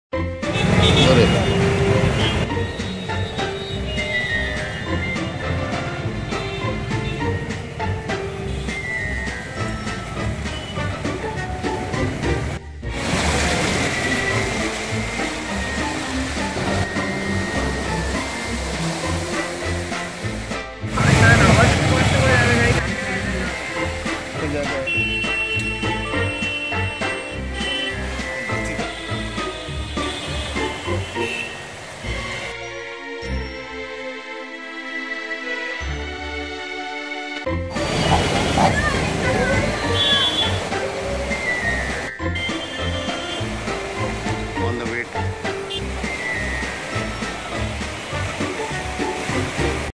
Flooded Kolkata Streets on 8th August,2016
While returning home at about 6 pm faced the flooded streets at Southern Avenue and Gulam Md.Sah Road near Golf Green.